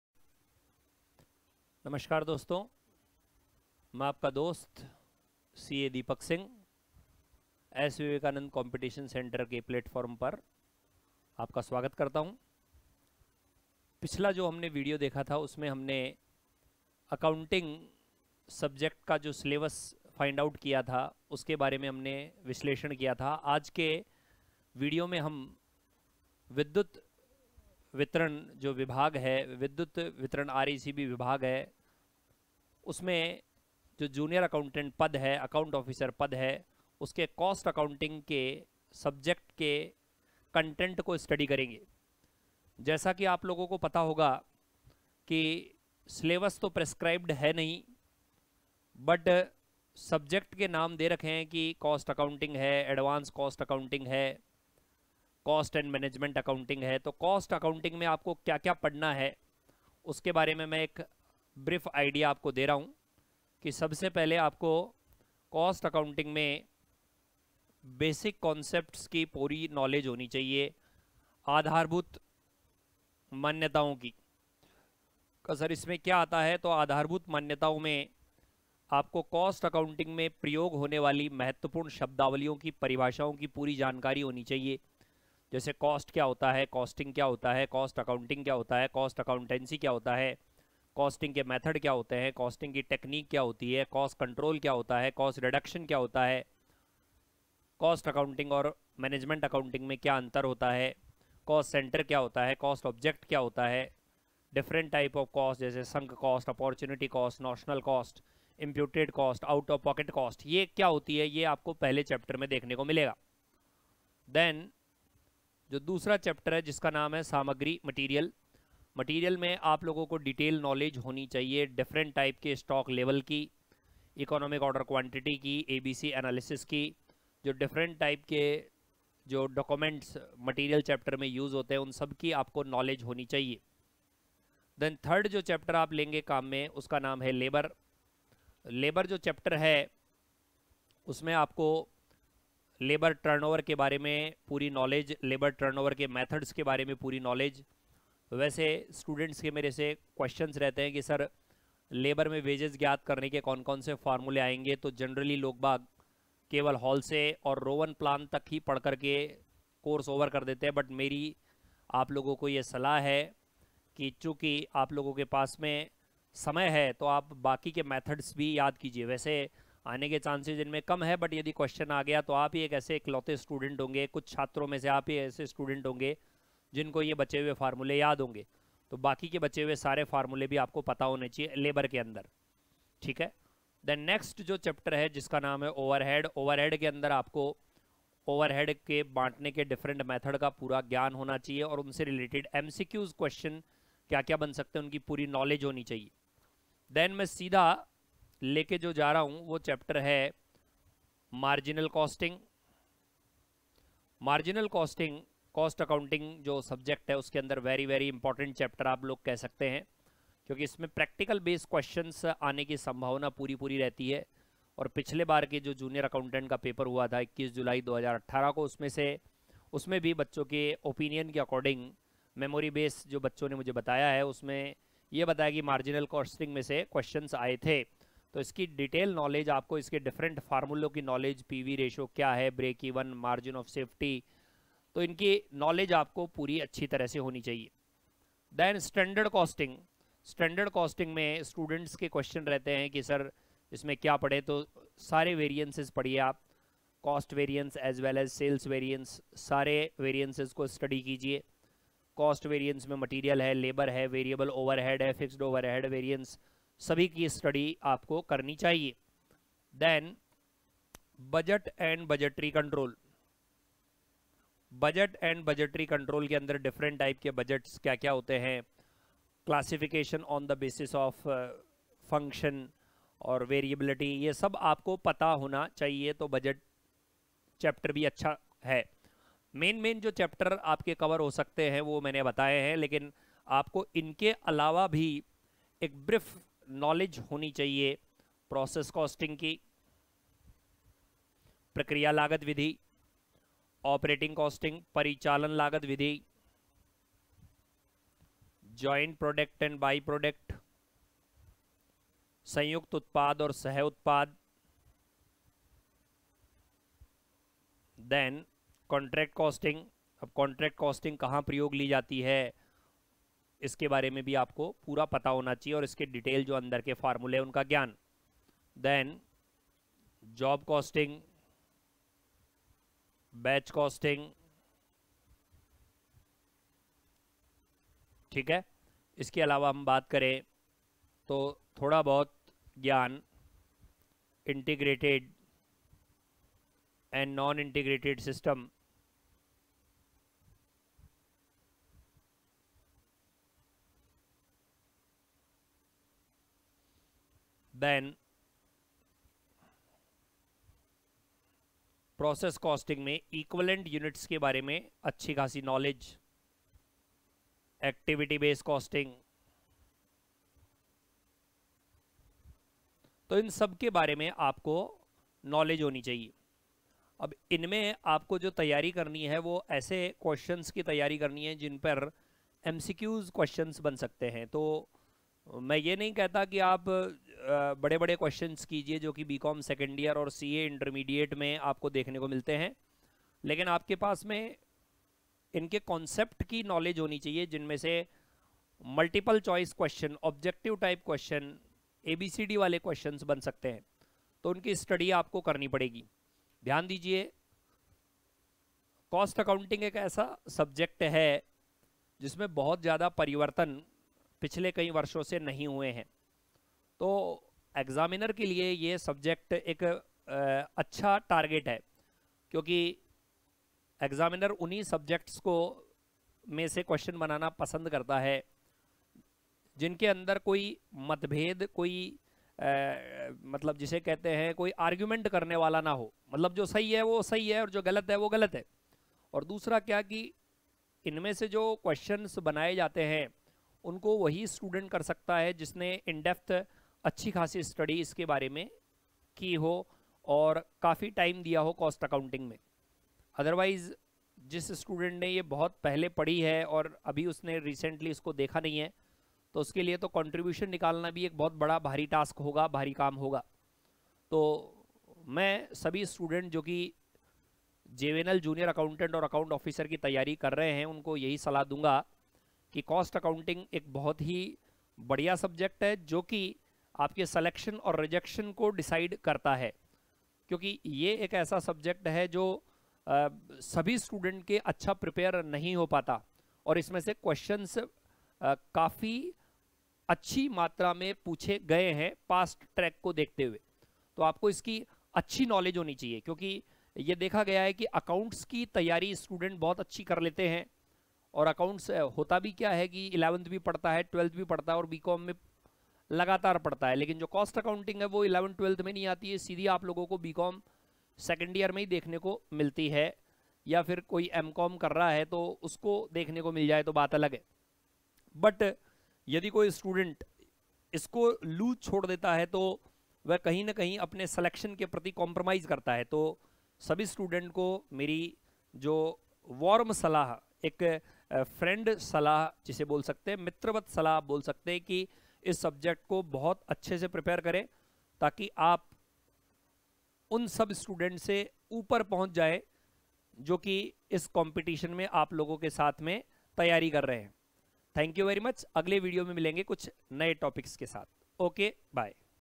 RSEB बिजली विभाग : JR ACCOUNTANT | Introduction of Cost Accounting | By - CA Deepak Singh
First ever Coaching Institute in Rajasthan, best known for record breaking results since 1985 in the field of different government exams like: